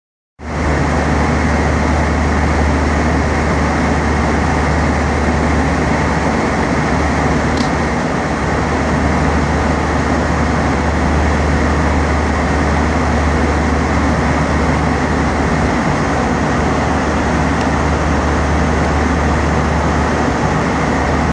Flying somewhere over the Rocky Mountains
Flying somewhere over the Rocky Mountains. Likely over Utah or Colorado.